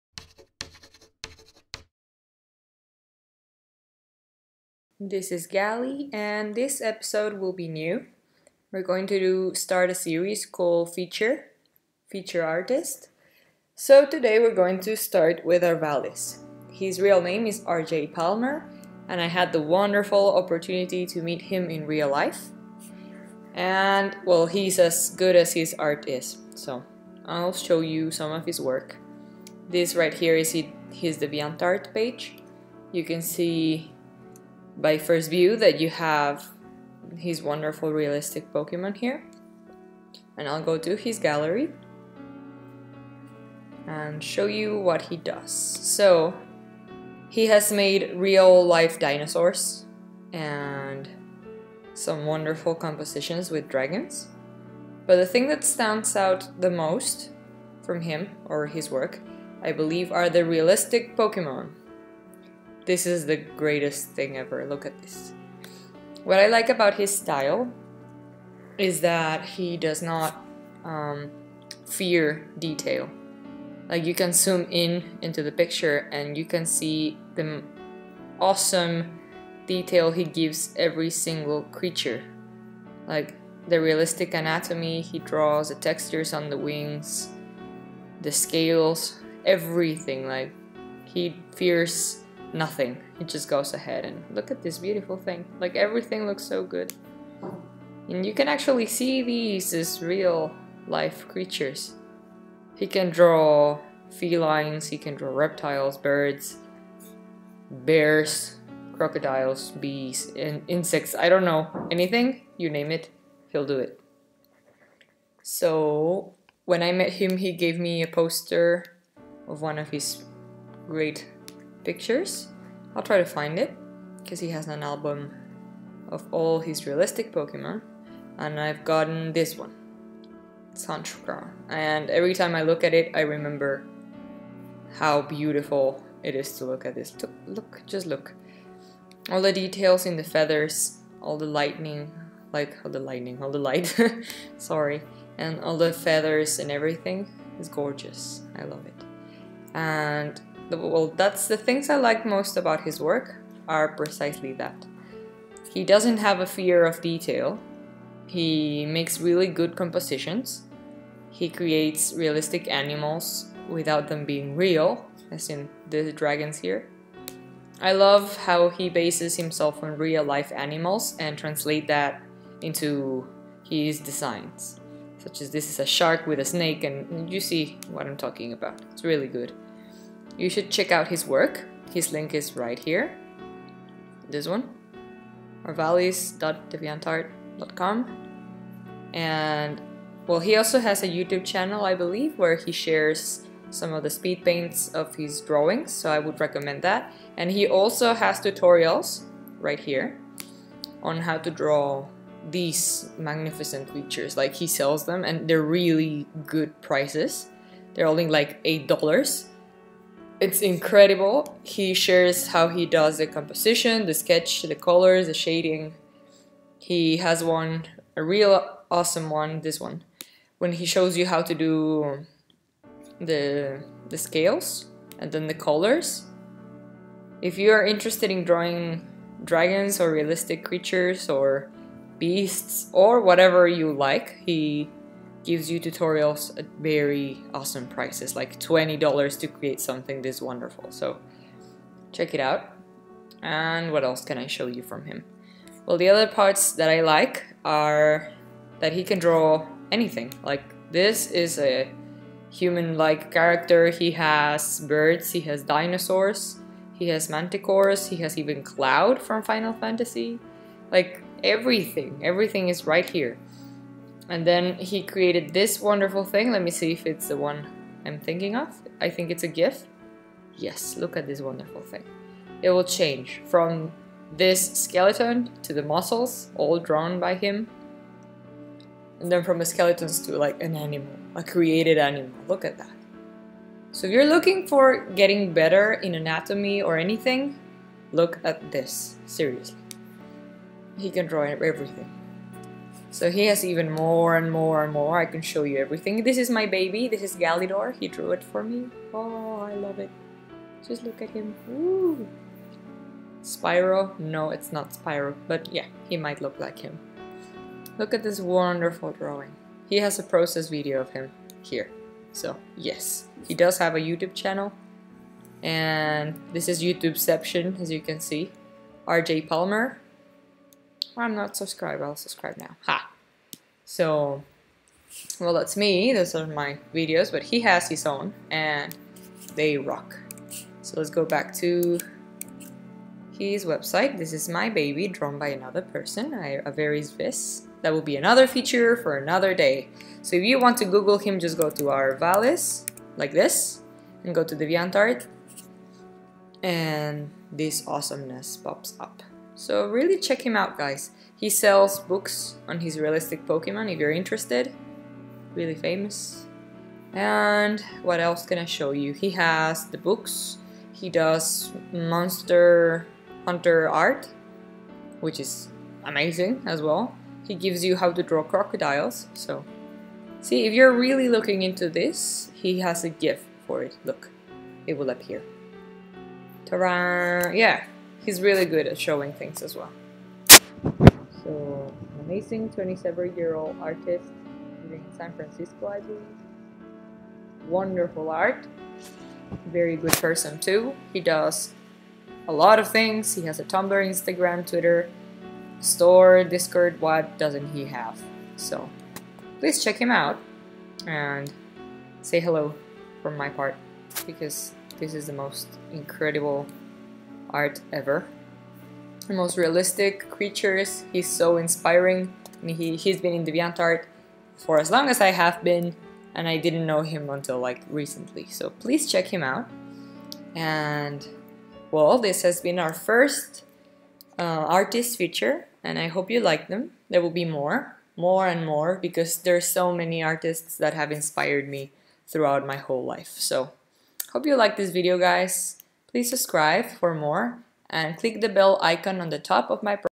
ARTIST FEATURE - Arvalis
I feature RJ Palmer, best known as Arvalis.